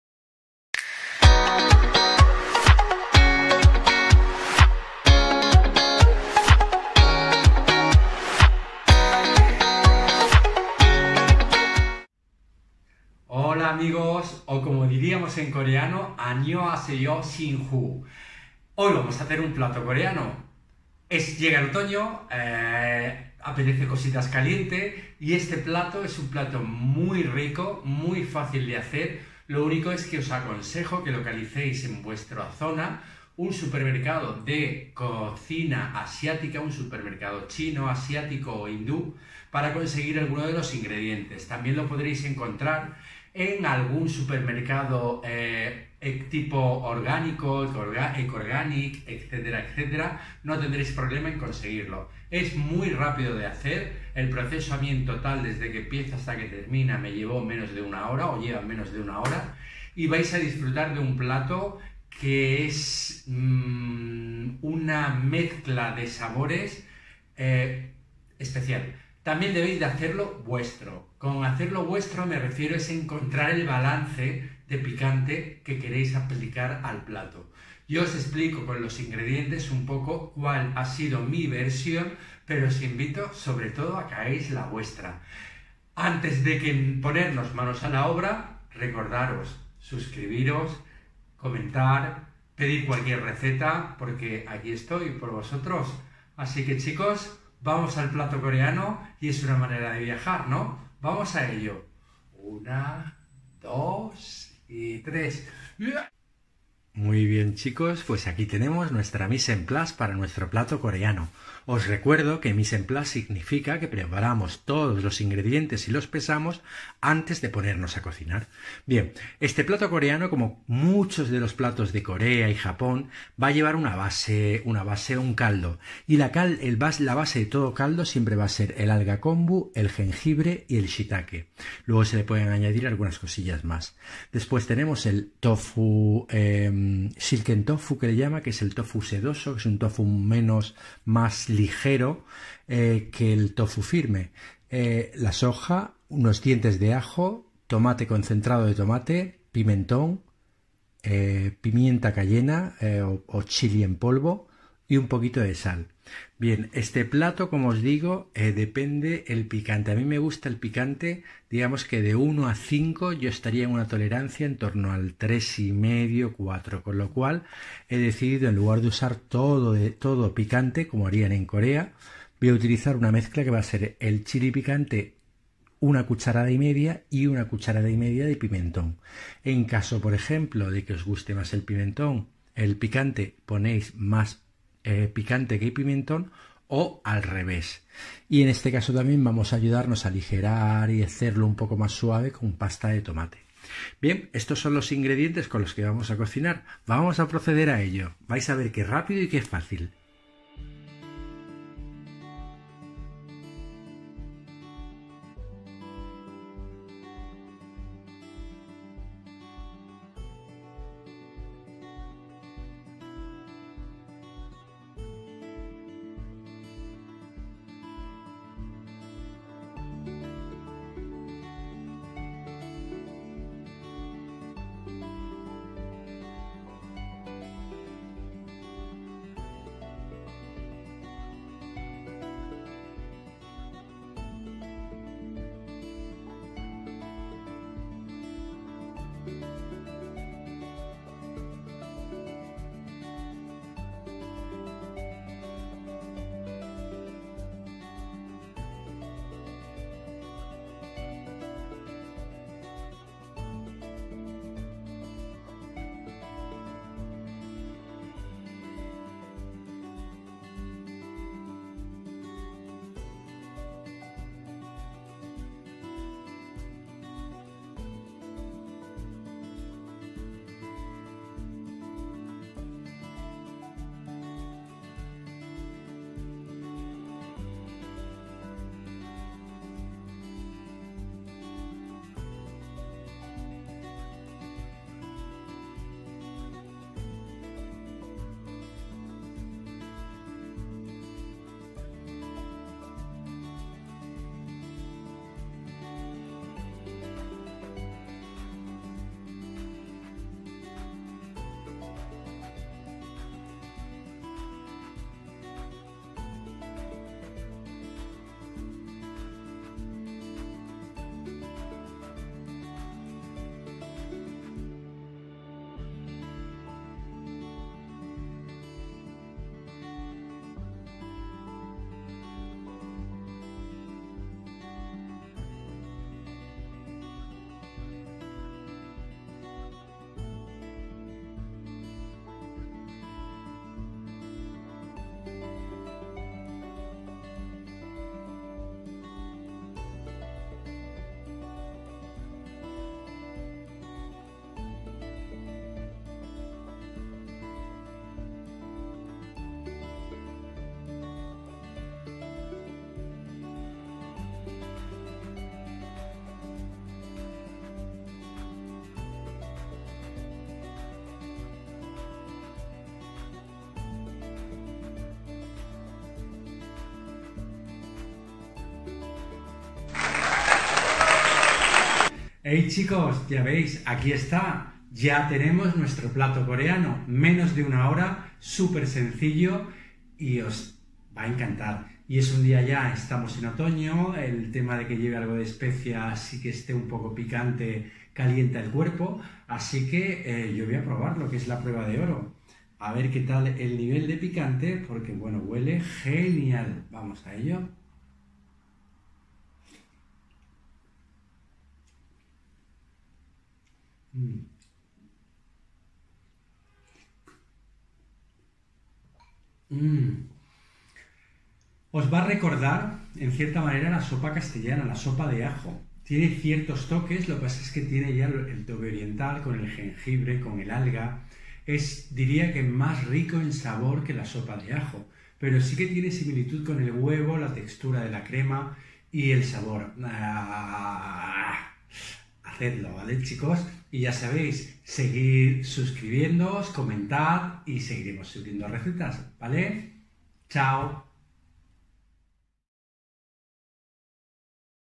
Plato koreano vegetariano. Sundubuu jjigae. FunEasyChef
Hoy traigo un plato Coreano. Es de la cocina tradicional Coreana, un plato de diario, fácil y muy rápido de preparar. En Corea se hace con chili picado pero para rebajar el picante yo adapto la receta usando menos chili y sustituyendo por pimentón y pasta de tomate. Os aconsejo que uséis la proporción de picante que mas se adapte a vuestro paladar, pero eso si, USAD PICANTE.
Los ingredientes los encontráis fácil en cualquier tienda de productos asiáticos, hindúes, japoneses. Las hay en casi todas las ciudades de mas de 100.000 habitantes y también los encontrareis en supermercado orgánicos. Para el otoño me parece un plato redondo. Muy rico en nutrientes y proteína y bajo en calorinas y carbohidratos. Probadlo.